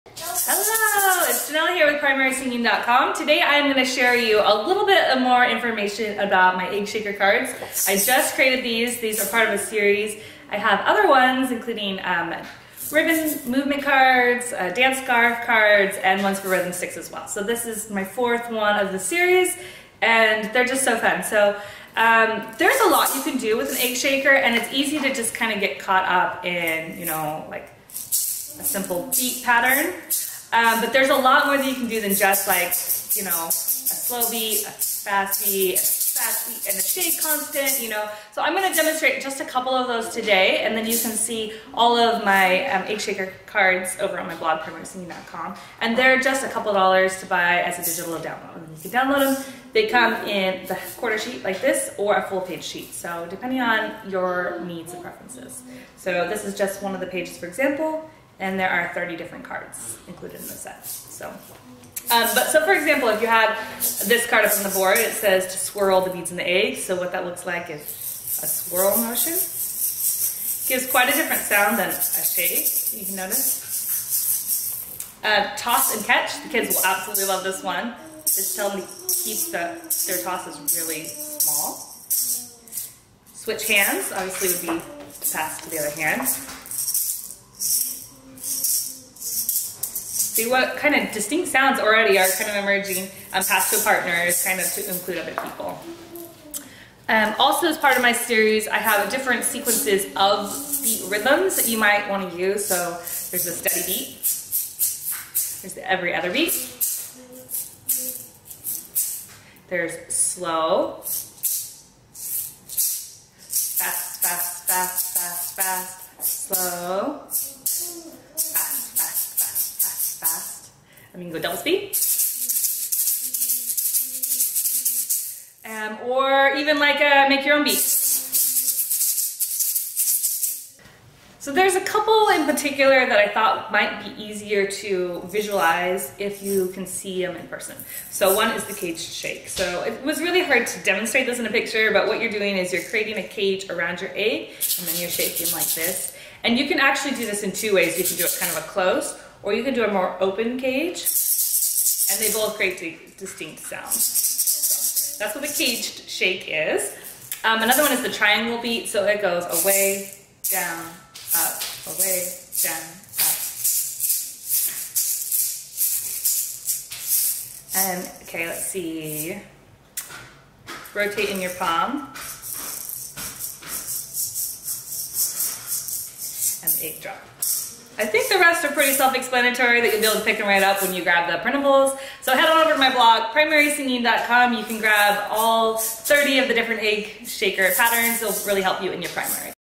Egg Shaker Beat Patterns
Have even more fun with music and singing time by using Egg Shakers! This video will demonstrate some unique ways to use egg shakers for creating different sounds and having fun with the egg shakers in a variety of way during music class or Primary.

Perfect for LDS Primary Music Leaders for Singing Time ideas and prep!